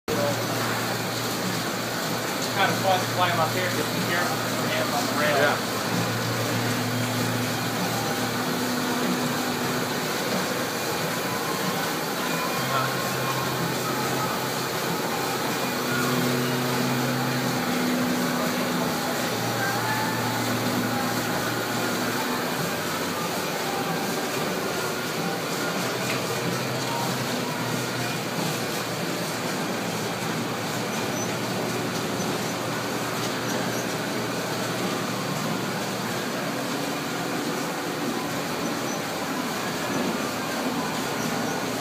Call of Cthulhu Poster Printing
Ever wonder how Call of Cthulhu posters are made? The HPLHS takes you behind the scenes to the big presses that get the job done.